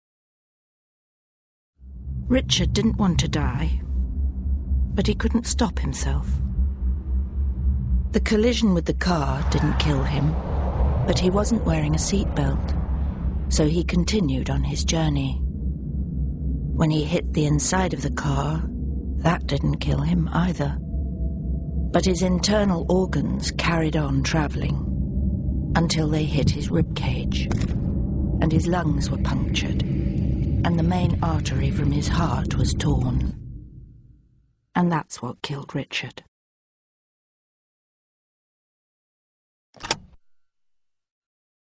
Внутренние органы при аварии
Видео показывает, что происходит с внутренними органами водителя, не пристёгнутого ремнями безопасности